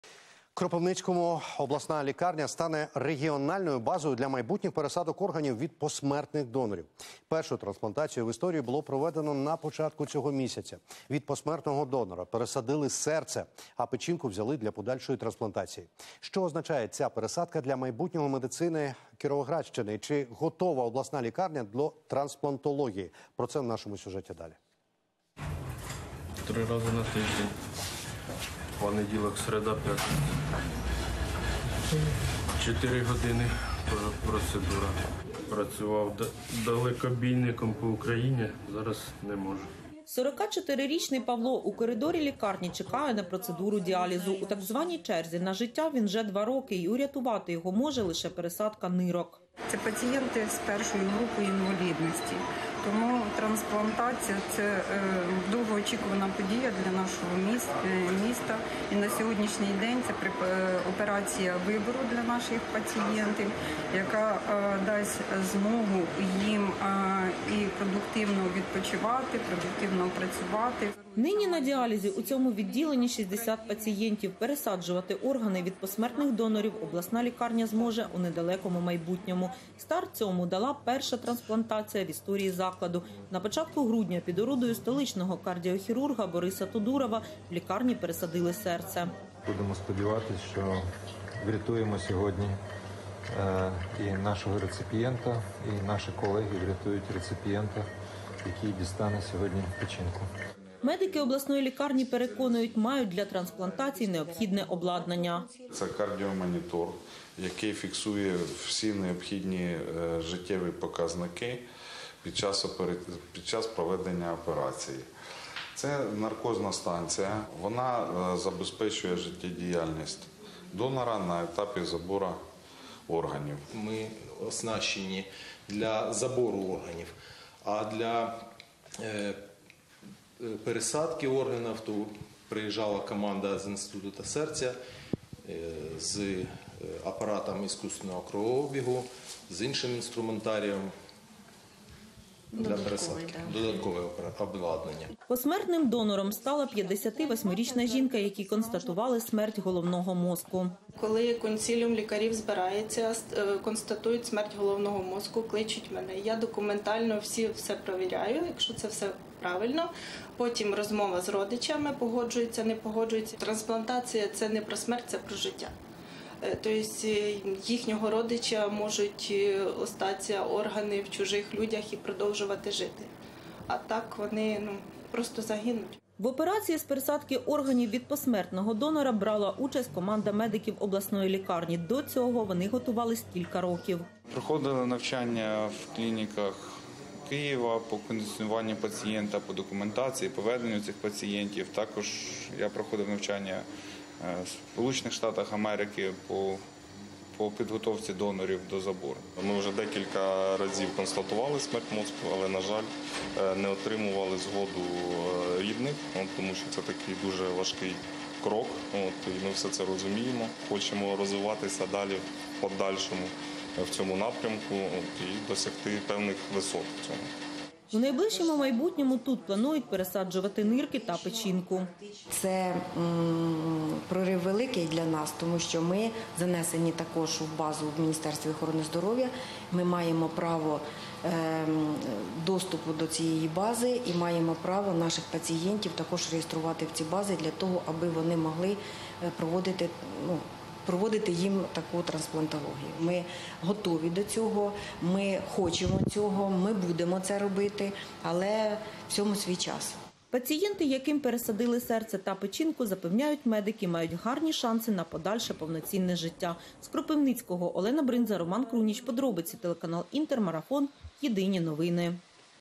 Кропивницька обласна лікарня стане регіональною базою для пересадок органів від посмертних донорів
Що означає ця пересадка для майбутнього медицини Кіровоградщини? Чи готова обласна лікарня до трансплантології - у нашому сюжеті.